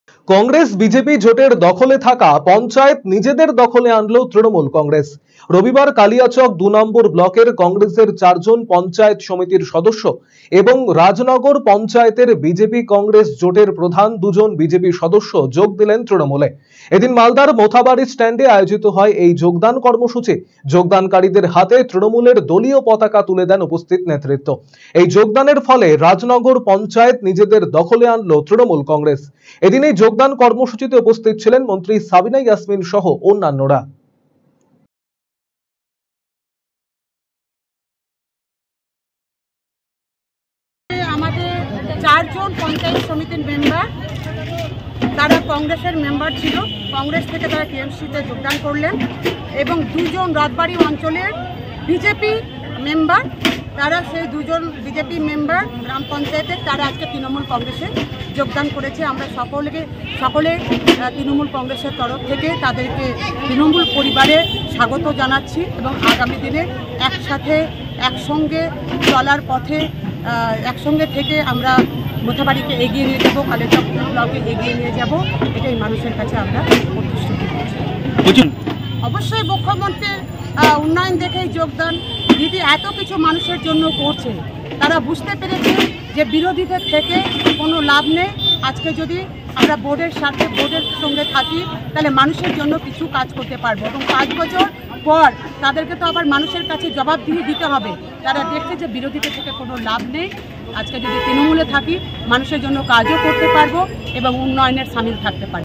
Tmc Joining | কংগ্রেস বিজেপি জোটের পঞ্চায়েত দখল তৃণমূলের
সংক্ষেপে খবরের বিষয়ঃ কংগ্রেস-বিজেপি জোটের দখলে থাকা পঞ্চায়েত নিজেদের দখলে আনলো তৃণমূল কংগ্রেস। রবিবার কালিয়াচক ২ ব্লকের কংগ্রেসের চারজন পঞ্চায়েত সমিতির সদস্য এবং রাজনগর পঞ্চায়েতের বিজেপি কংগ্রেস জোটের প্রধান দুজন বিজেপি সদস্য যোগ দিলেন তৃণমূলে।